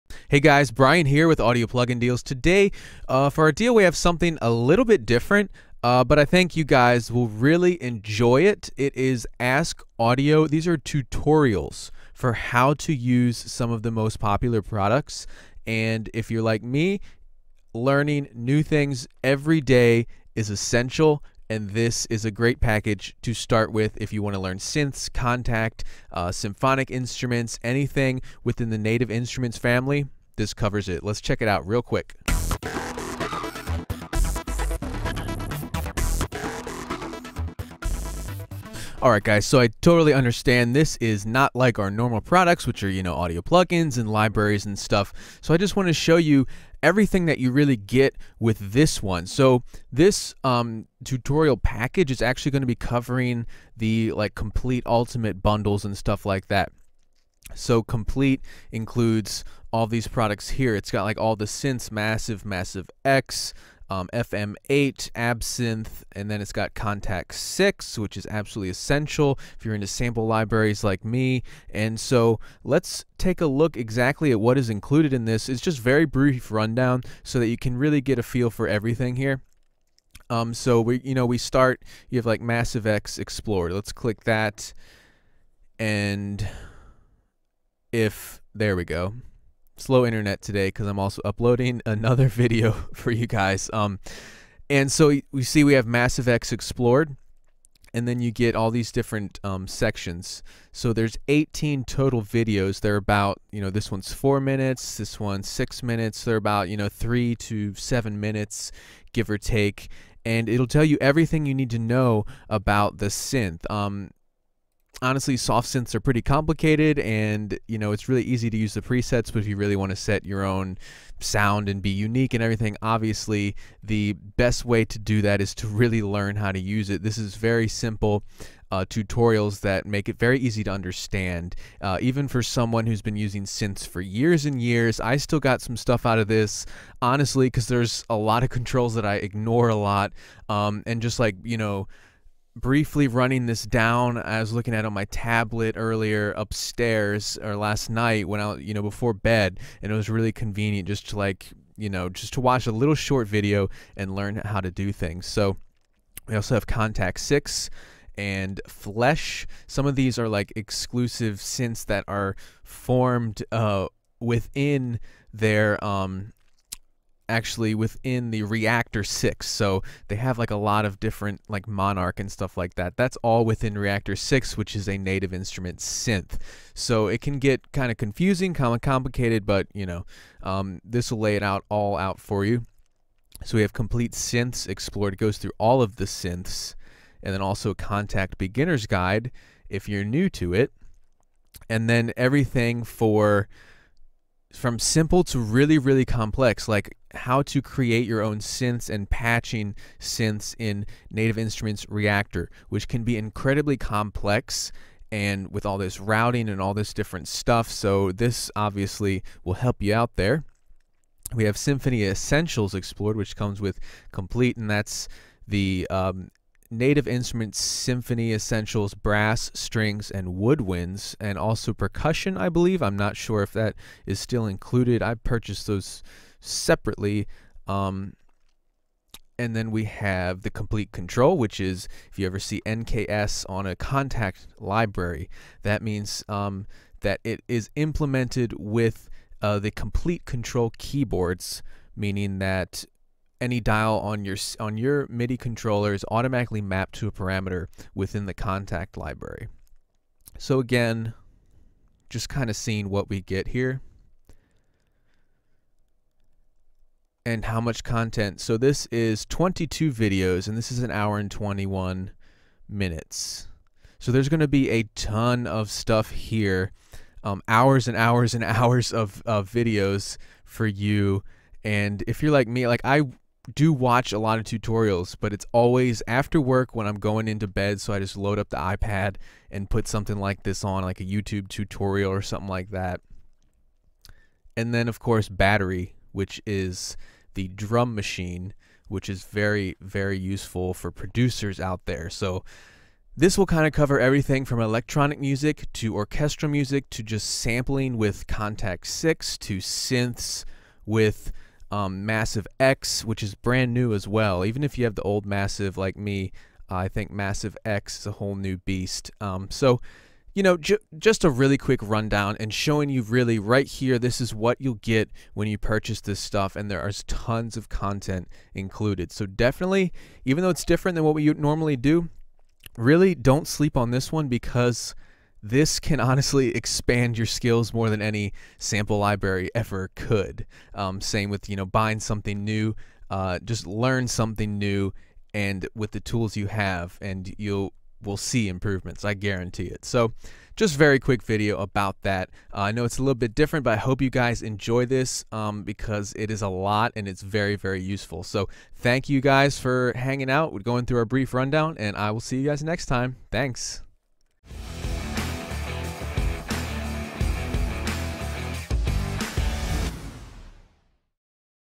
Native Instruments KOMPLETE TRAINING COURSES BUNDLE by Ask Audio Academy GET 80% OFF
Okay guys, this deal is a bit different than anything we normally offer, BUT, I truly think you will all be able to get a lot out of it. This deal is for a bundle of tutorial videos from Ask Audio, covering every aspect of Native Instruments Komplete bundle, which means everything from Kontakt to Massive X and everything in between.